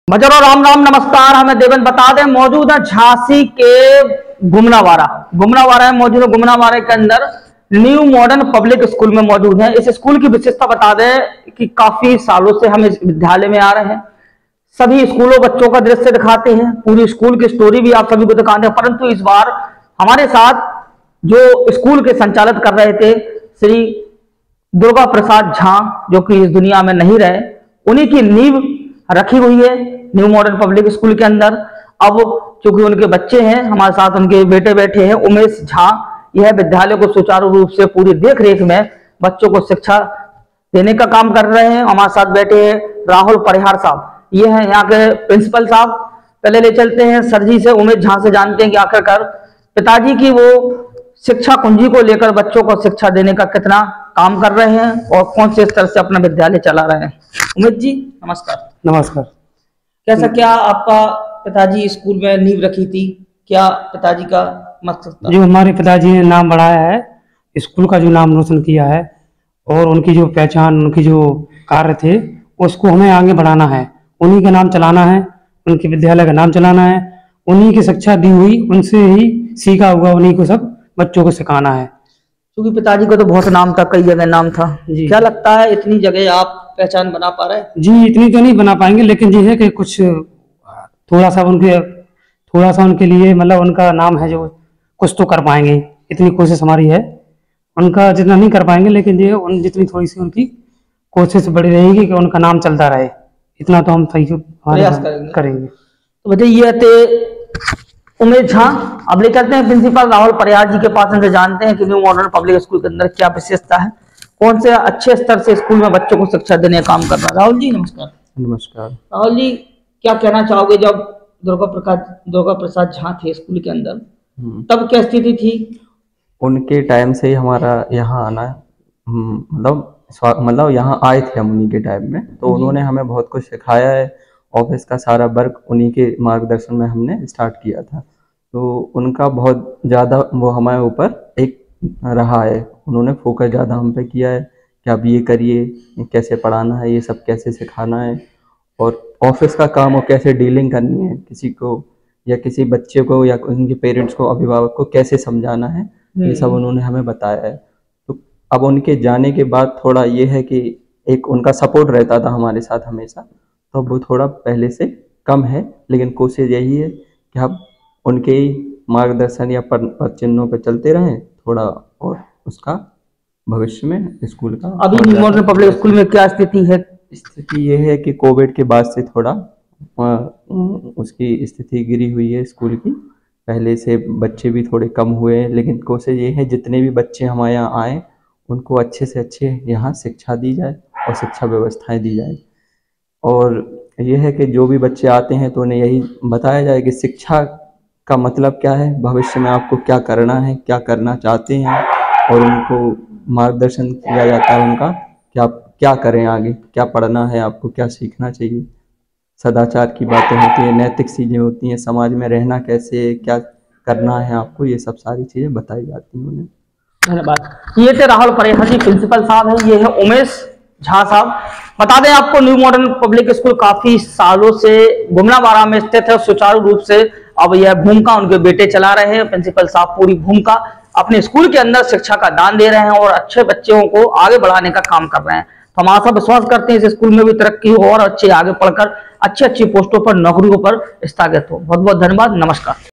गुमनावारा में स्व० दुर्गाप्रसाद झा ने रखी नीव न्यू मॉर्डन पब्लिक स्कूल को बेटे उमेश झा कर रहे देखभाल
गुमनावारा में स्व० दुर्गाप्रसाद झा ने रखी नीव न्यू मॉर्डन पब्लिक स्कूल को बेटे उमेश झा कर रहे देखभाल dev news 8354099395